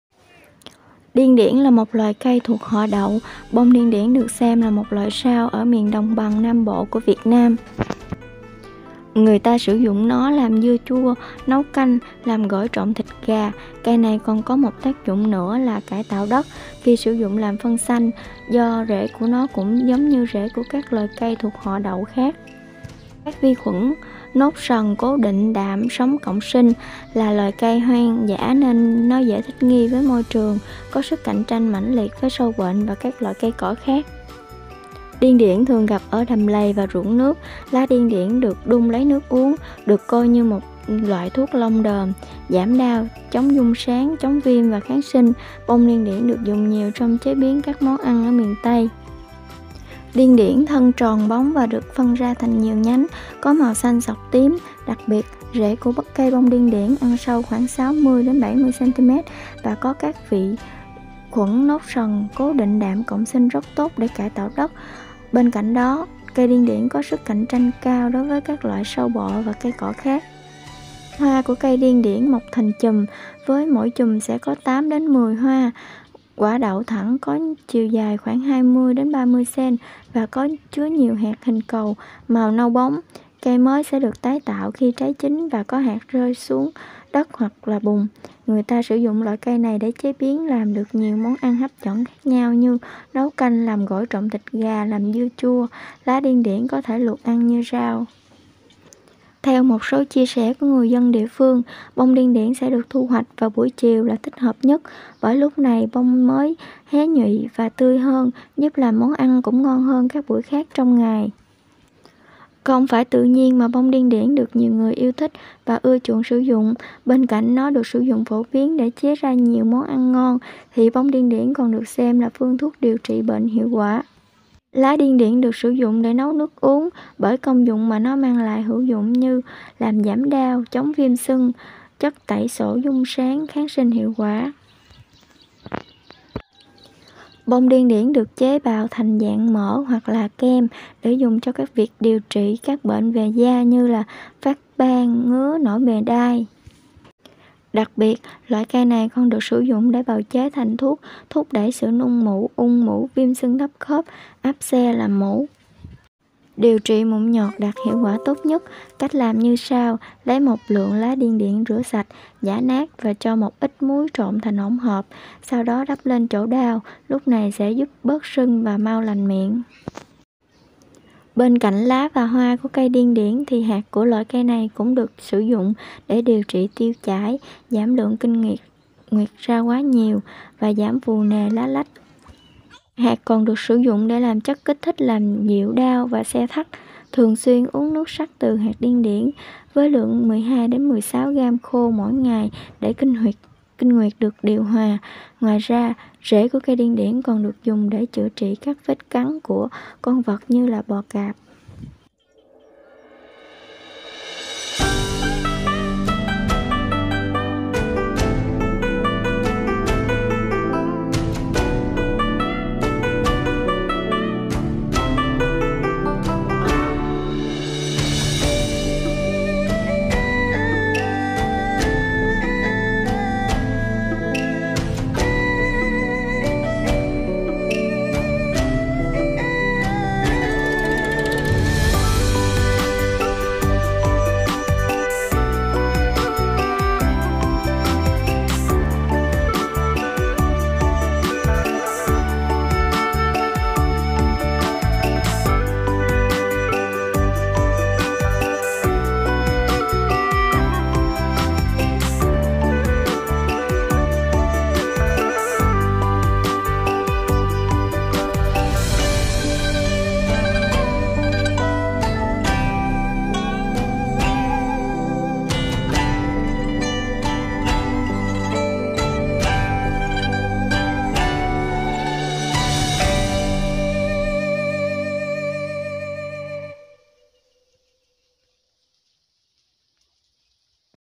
Những Lợi Ích Của BÔNG ĐIÊN ĐIỂN Có Thể Bạn Chưa Biết / Trang TiDi #70
Điên điển là một loài cây thuộc họ Đậu. Bông điên điển được xem là một loại rau ở miền đồng bằng Nam Bộ của Việt Nam. Người ta sử dụng nó làm dưa chua, nấu canh, làm gỏi trộn thịt gà. Cây này còn một tác dụng nữa là cải tạo đất khi sử dụng làm phân xanh, do rễ của nó cũng giống như rễ của các loài cây thuộc họ Đậu khác, có các vi khuẩn nốt sần cố định đạm sống cộng sinh. Là loài cây hoang dã nên nó dễ thích nghi với môi trường, có sức cạnh tranh mãnh liệt với sâu bệnh và các cây cỏ khác.
Điên điển thường gặp ở đầm lầy và ruộng nước. Lá điên điển được đun lấy nước uống, được coi như một loại thuốc long đờm, giảm đau, chống giun sán, chống viêm và kháng sinh. Bông điển điển được dùng nhiều trong chế biến các món ăn miền Tây
Điên điển thân tròn bóng và được phân ra thành nhiều nhánh, có màu xanh sọc tím. Đặc biệt, rễ của cây Bông Điên Điển ăn sâu khoảng 60 – 70cm và có các vị khuẩn nốt sần cố định đạm cộng sinh rất tốt để cải tạo đất làm phân xanh. Bên cạnh đó, cây Điển Điển có sức cạnh tranh cao đối với các loại sâu bọ và cây cỏ khác.
Hoa của cây Điên Điển mọc thành chùm với mỗi chùm sẽ có 8 – 10 hoa to. Quả đậu thẳng có chiều dài khoảng 20 đến 30 cm và chứa nhiều hạt có hình cầu, màu nâu bóng. Cây mới sẽ được tái tạo khi trái chín và có hạt rớt xuống đất hoặc bùn. Người ta sử dụng loại cây này để chế biến làm được nhiều món ăn hấp dẫn khác nhau như nấu canh, làm gỏi trộn thịt gà, làm dưa chua, lá Điên Điển có thể luộc ăn như rau….
Theo một số chia sẻ của người dân địa phương, Bông Điên Điển sẽ được thu hoạch vào buổi chiều là thích hợp nhất. Bởi lúc này bông chỉ mới hé nhụy và tươi hơn giúp làm món ăn cũng ngon hơn các buổi khác trong ngày.
Không phải tự nhiên mà Bông Điên Điển được nhiều người yêu thích và ưa chuộng sử dụng. Bên cạnh nó được sử dụng phổ biến để chế biến ra nhiều món ăn ngon thì Bông Điên Điển còn được xem là phương thuốc điều trị bệnh hiệu quả:
• Lá Điên Điển được sử dụng để nấu nước uống. Bởi công dụng mà nó mang lại hữu dụng như làm giảm đau, chống viêm sưng, chất tẩy xổ giun sán, kháng sinh hiệu quả…
• Bông Điên Điển được chế bào thành dạng mỡ hoặc kem để dùng cho việc điều trị các bệnh về da như phát ban, ngứa, nổi mề đay.
• Đặc biệt, loại cây này còn được sử dụng để bào chế thành thuốc thúc đẩy sự nung mủ, ung mủ, viêm sưng thấp khớp, áp – xe, làm mủ
• Điều trị mụn nhọt đạt hiệu quả tốt nhất, với cách làm như sau: Lấy một lượng lá Điên Điển rửa sạch, giã nát và cho một ít muốn trộn thành hỗn hợp sau đó đắp lên chỗ đau. Lúc này sẽ giúp bớt sưng và mau lành miệng.
• Bên cạnh lá và hoa của cây Điên Điển thì hạt của loại cây này cũng được sử dụng để điều trị tiêu chảy, giảm lượng kinh nguyệt ra quá nhiều và giảm phù nề lá lách.
• Hạt còn được sử dụng để làm chất kích thích làm dịu đau và se thắt.
• Thường xuyên uống nước sắc từ hạt Điên Điển với lượng 12 – 16g khô mỗi ngày để kinh nguyệt được điều hòa.
• Ngoài ra, rễ của cây Điên Điển còn được dùng để chữa trị các vết cắn của con vật như bò cạp.

Cảm ơn mọi người đã xem video của mình, nếu thấy hay hãy cho mình 1 like subscribe kênh của mình nhé, để cho mình có động lực làm những video hay hơn ạ!